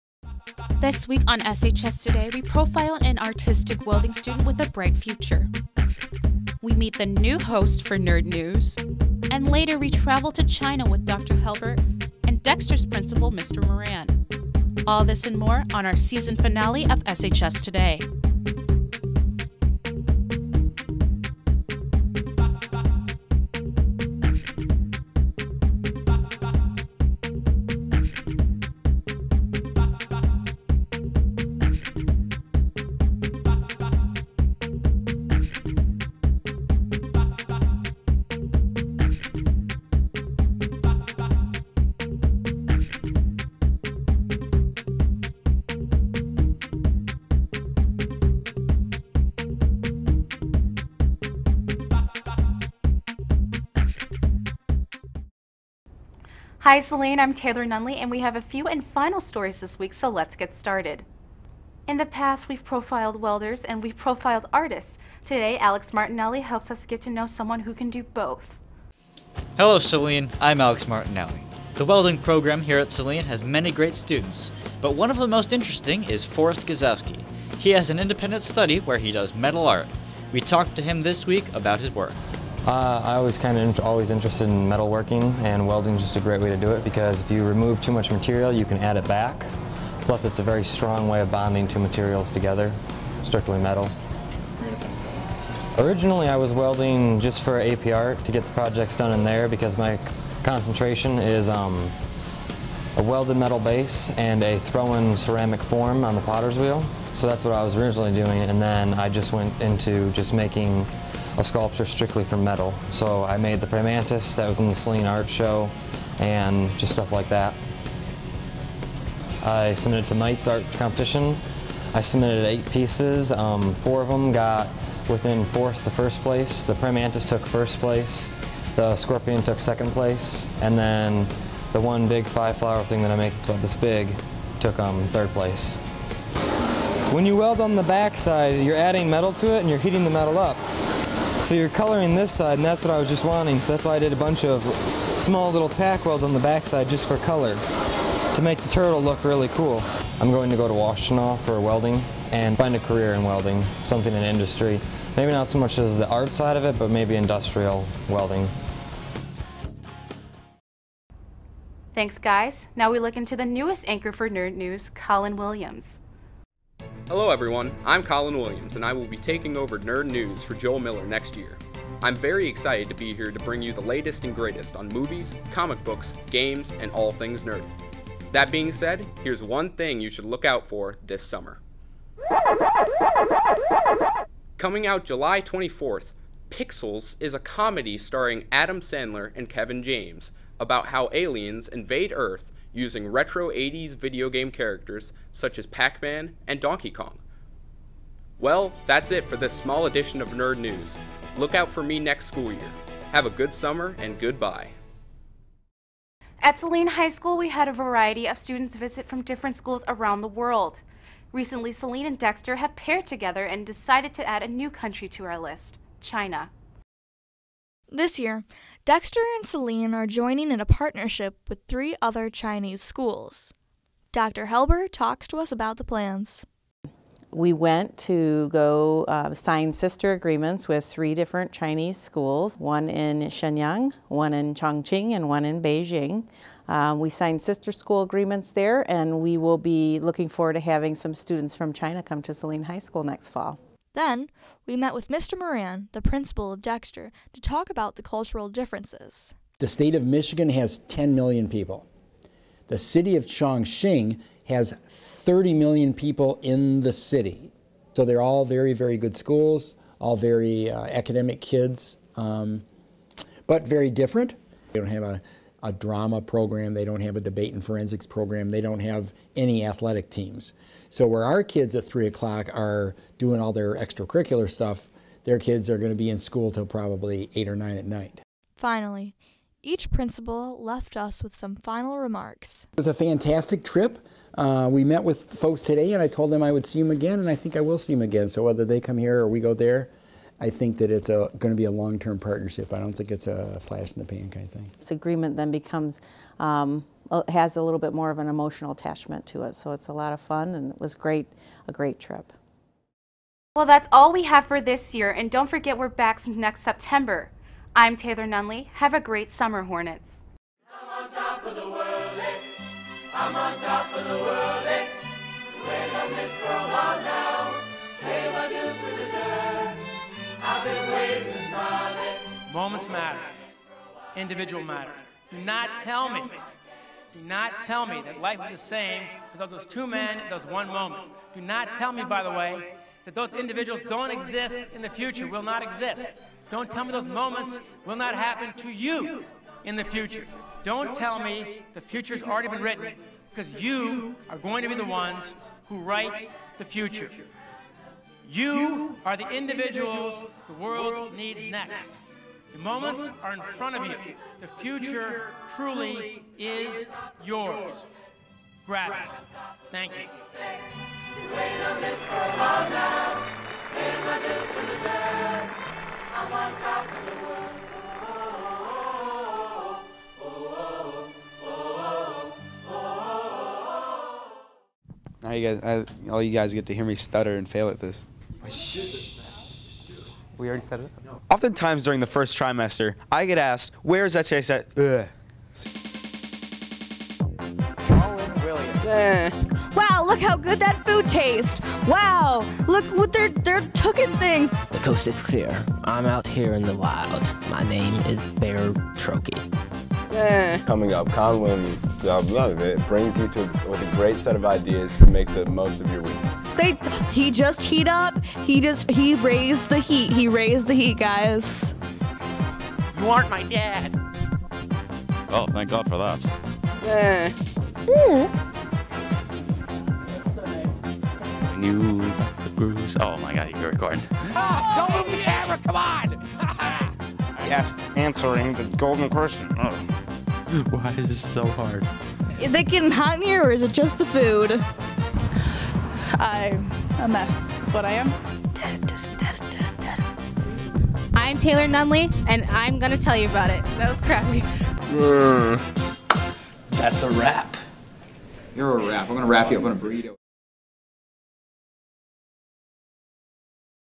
SHS Today 6.5 - Season Finale
SHS Today - June 5, 2015

Host:
Taylor Nunlee

Features:
Forest G. - Artistic Welder
Chinese Foreign Exchange 15-16
Nerd News
Blooper Reel
Bonus - Graduation Highlights

Thank you for an amazing season. Look for us to return in September.